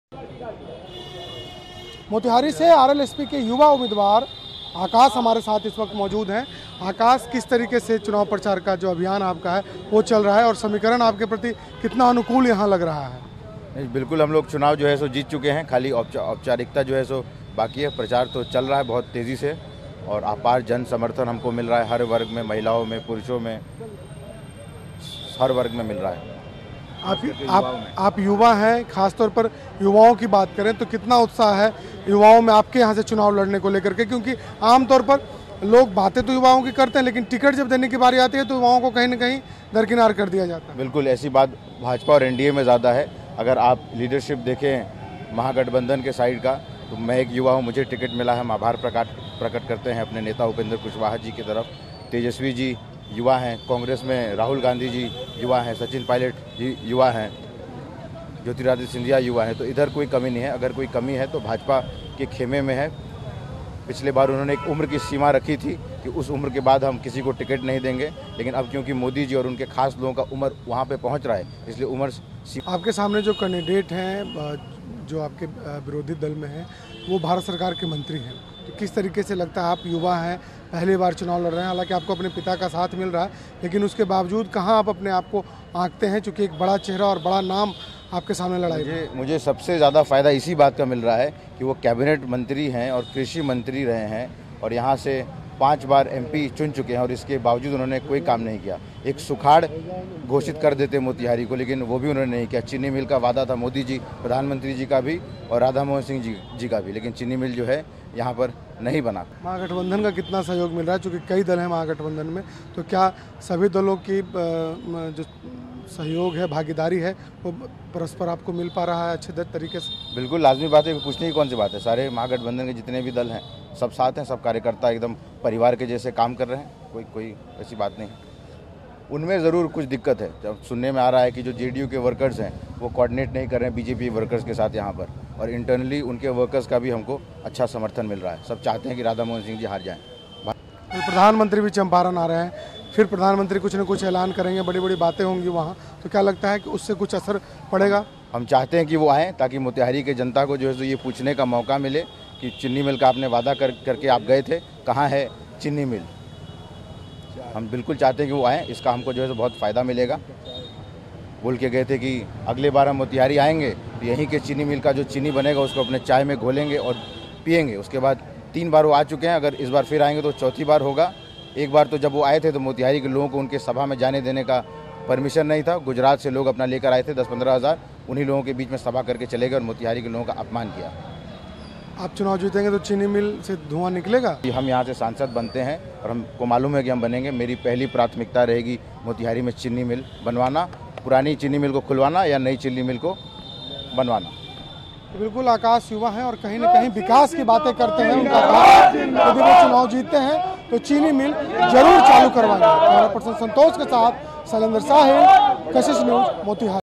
Bihar के सबसे कम उम्र के लोकसभा प्रत्याशी हैं Akash Singh, देखिए जीत के बारे में क्या बोले
Bihar के सबसे कम उम्र के लोकसभा प्रत्याशी ,  RLSP से मोतिहारी सीट से लड़ रहे हैं चुनाव , देखें क्या बोले जीत के बारे में